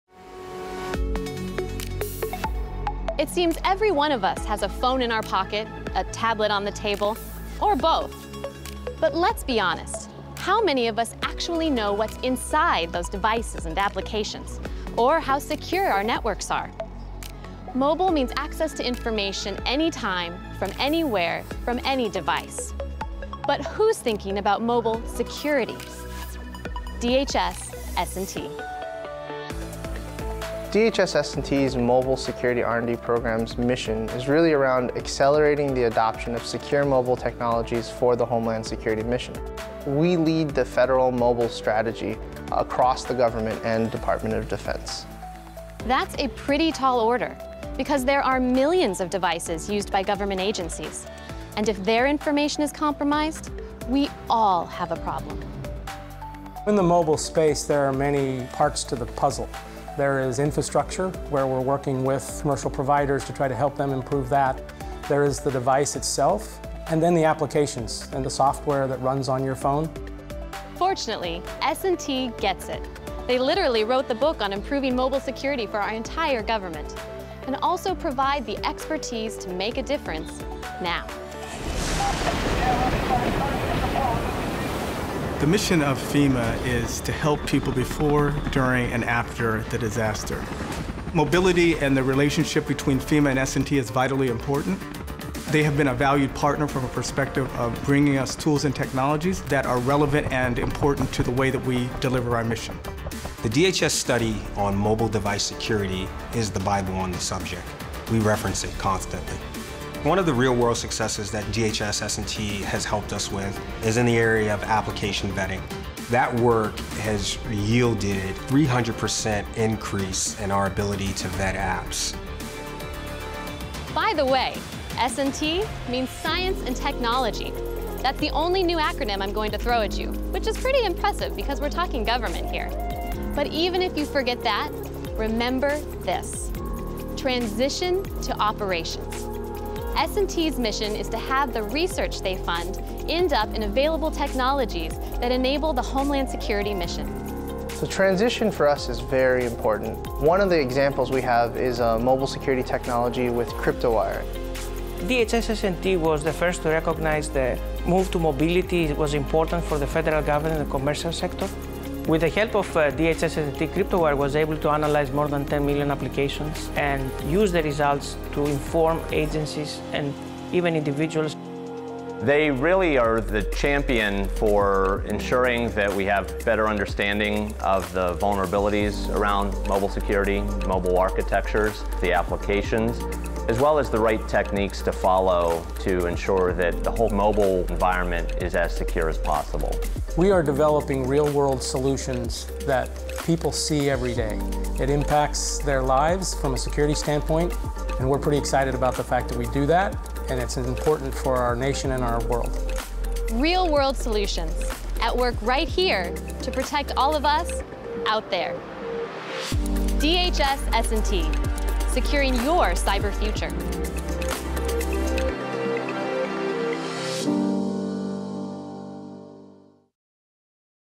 Mobile Security
The Department of Homeland Security (DHS) workforce has become increasingly mobile, driving the need for secure mobility solutions and a coordinated approach and framework to guide the selection and implementation of common enterprise mobility solutions. To accelerate the safe and secure adoption of mobile technology within DHS and the federal government, the DHS Science and Technology Directorate (S&T) created the Mobile Security research and development (R&D) Program.

The Mobile Security R&D program has three efforts address in  R&D: Mobile Device Security, Mobile Application Security and Mobile Network Infrastructure.